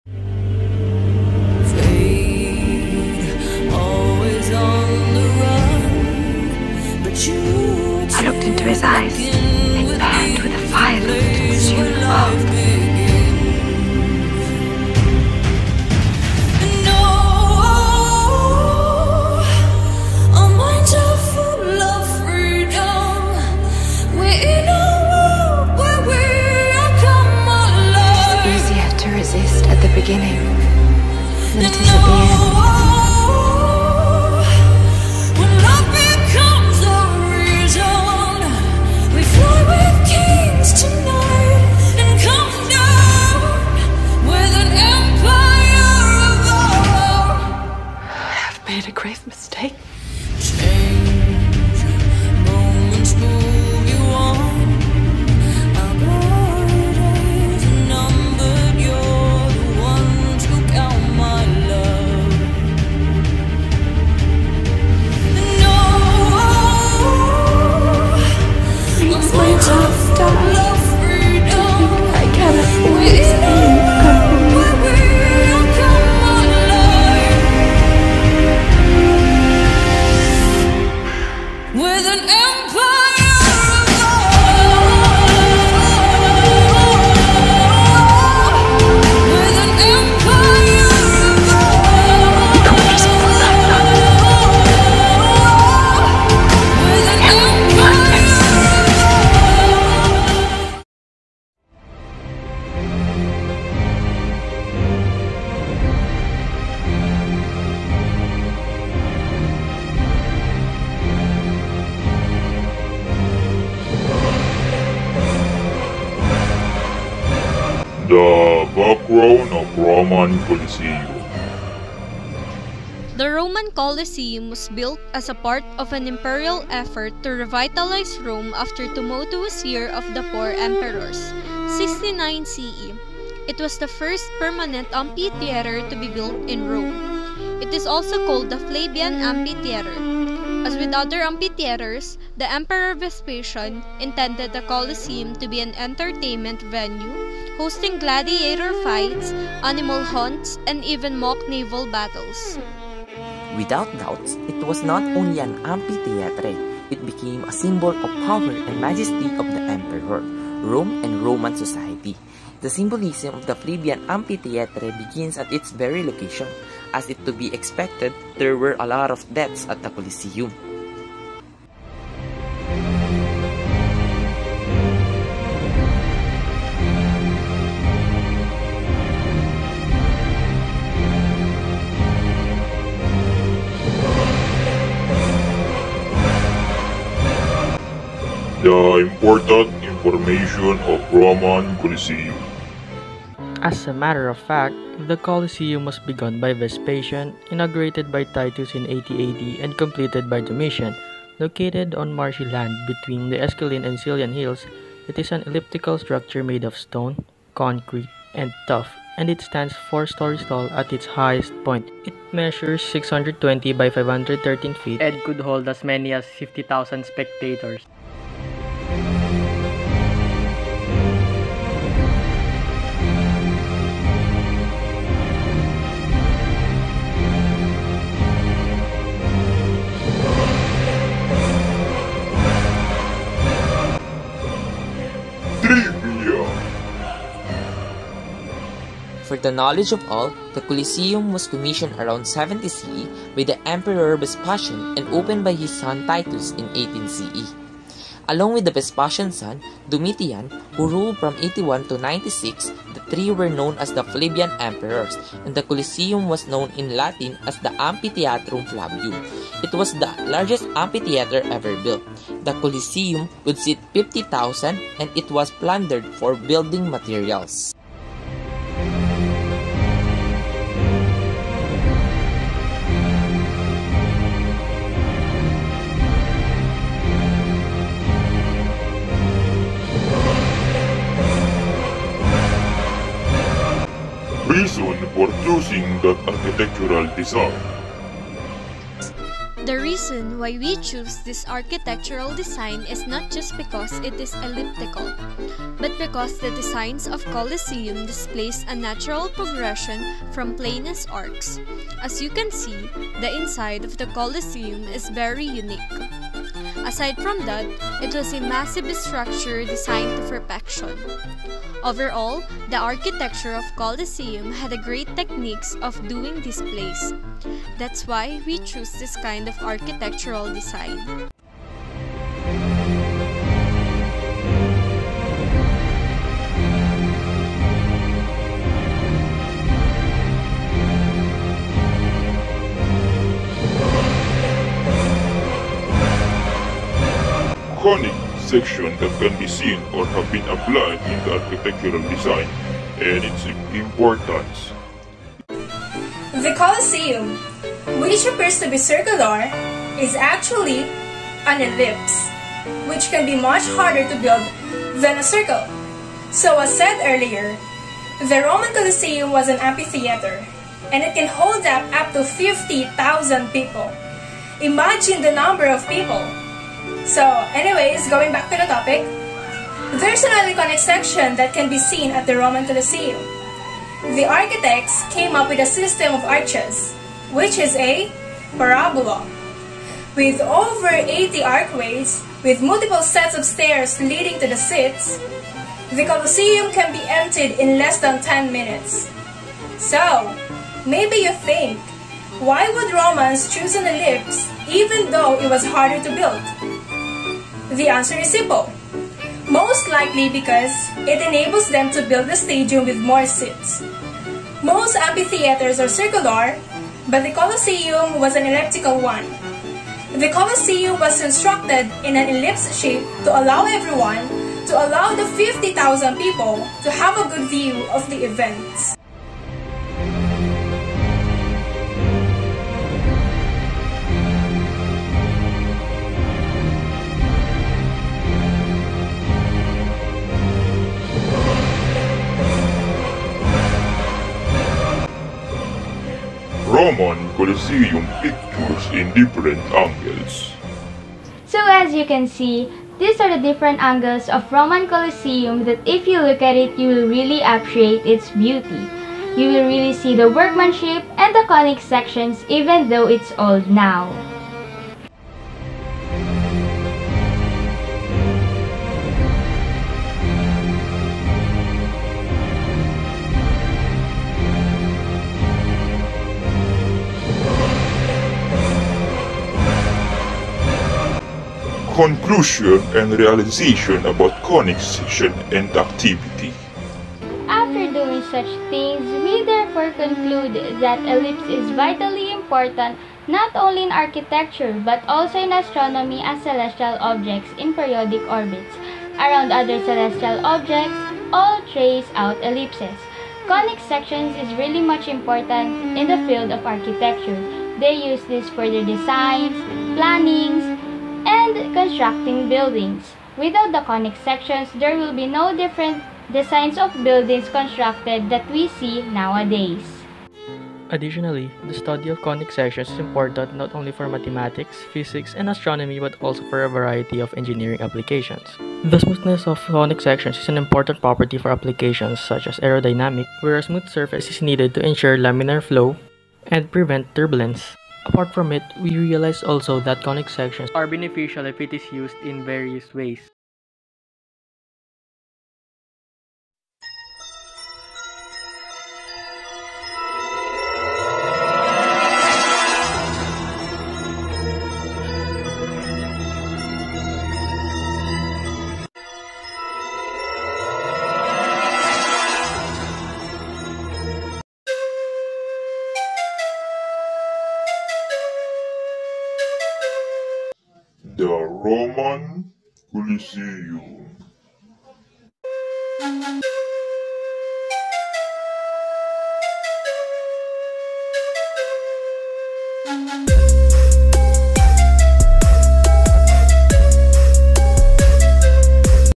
11 - CHRONICLES [GROUP 3] - Roman Colosseum
ACTIVITY:
Conic Sections: A Virtual Presentation of Real-World Architectural Designs
This activity will serve as one of the final outputs for second semester in STEM 001 – Precalculus. The performance standard of the course states that the learner shall be able to model situations appropriately and solve problems accurately using conic sections and systems of nonlinear equation.